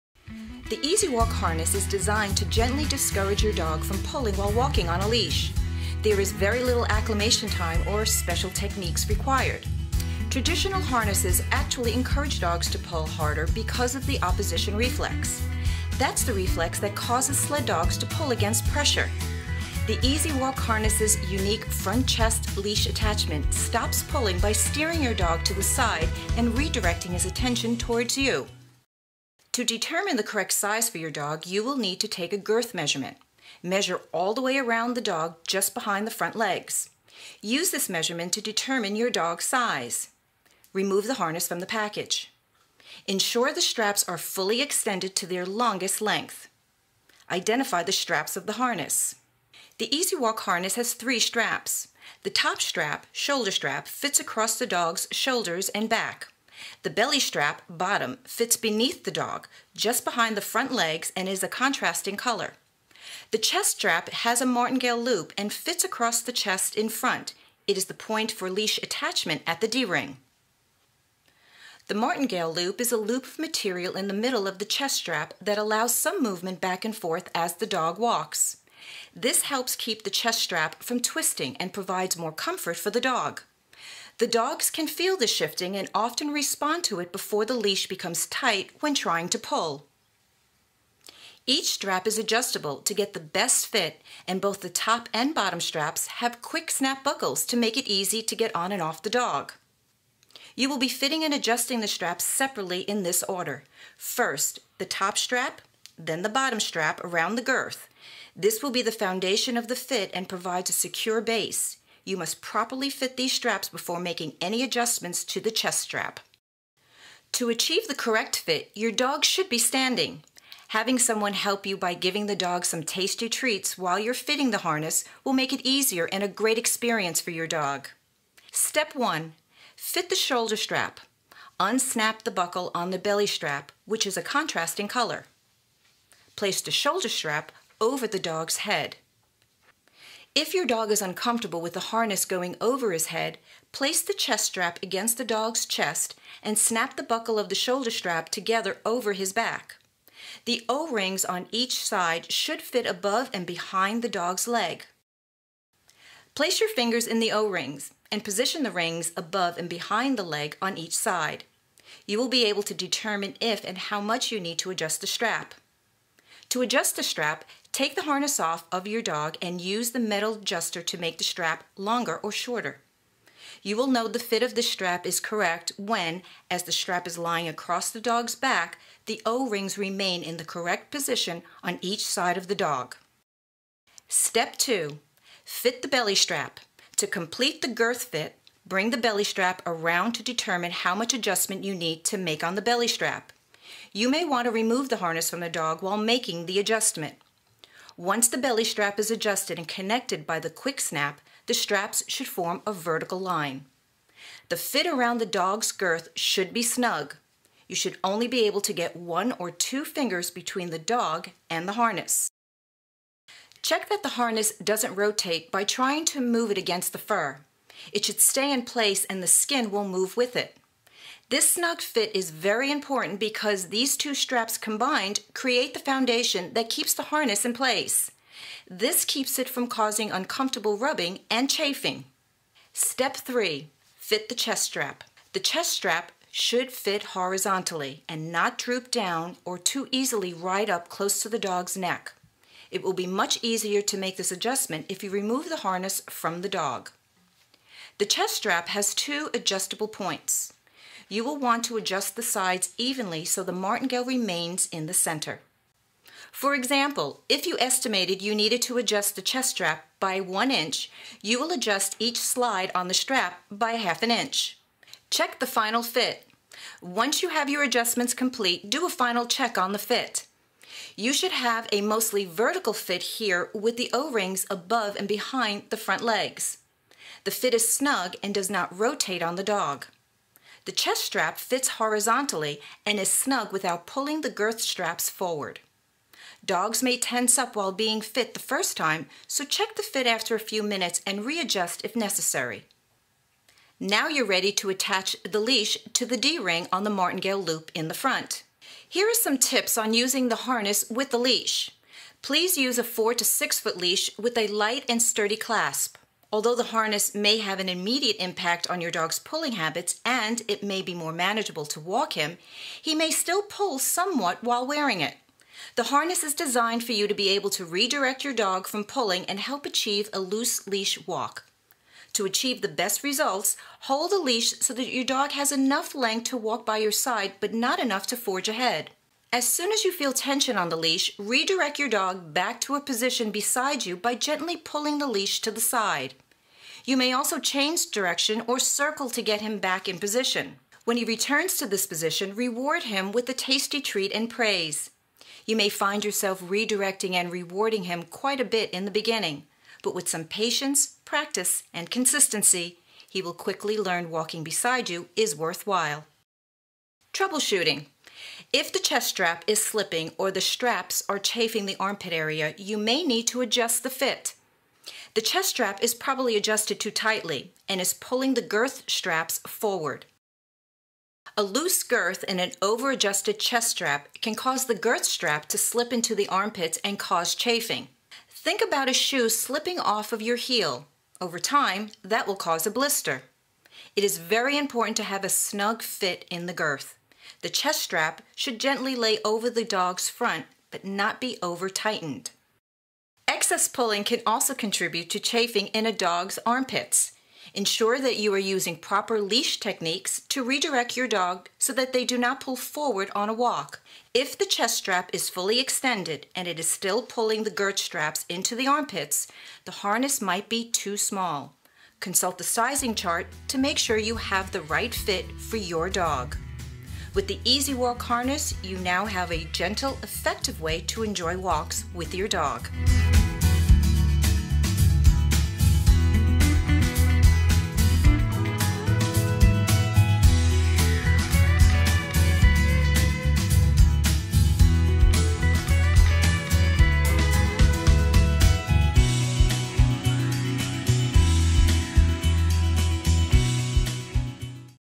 How to Fit and Use the EasyWalk® Dog Harness - PetSafe®
The EasyWalk® Harness is designed to gently discourage dogs from pulling while walking on a lead. This video shows how to properly fit and adjust your harness for the perfect fit for your dog. Recommended by vets and trainers, they're suitable for all types of dog and are available in a choice of sizes and colours.

*Harness featured in the video (Royal Blue/Navy) is not available in all regions.
**The number in the video is for U.S. only. For UK customer support please Contact UK Customer Care on freephone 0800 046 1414 with any further questions.